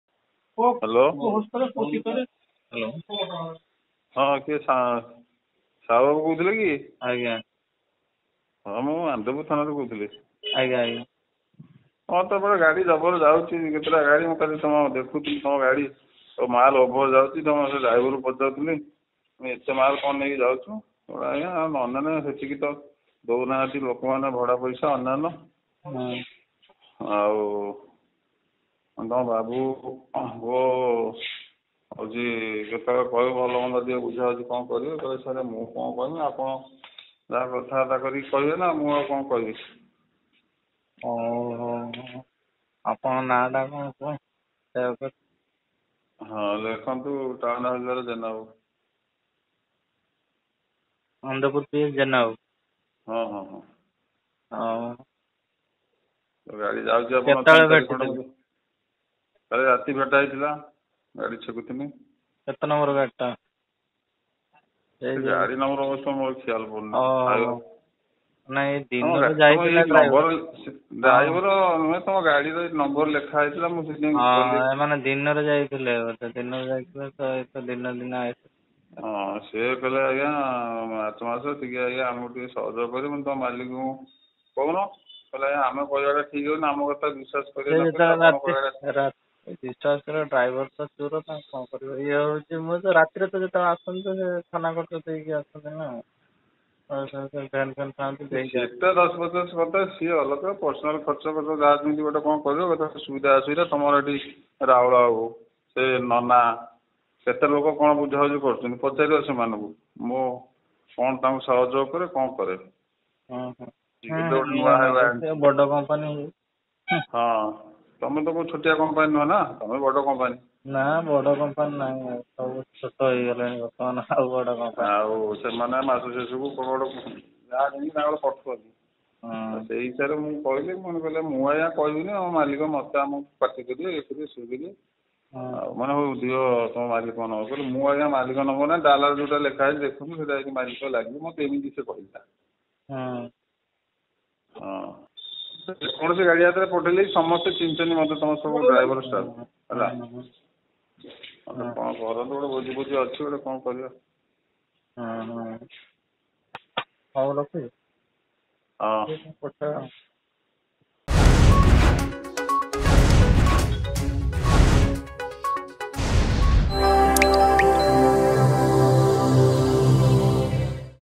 ଭାଇରାଲ ହେଲା ବଟି ମାଗିବାର ଅଡିଓ...
ବାଲି ଗାଡି ଡ୍ରାଇଭରକୁ ବଟି ମାଗି ଅଡୁଆରେ ପୋଲିସ କନେଷ୍ଚବଳ. ଭାଇରାଲ ହେଲା ବଟି ମାଗିବାର ଅଡିଓ...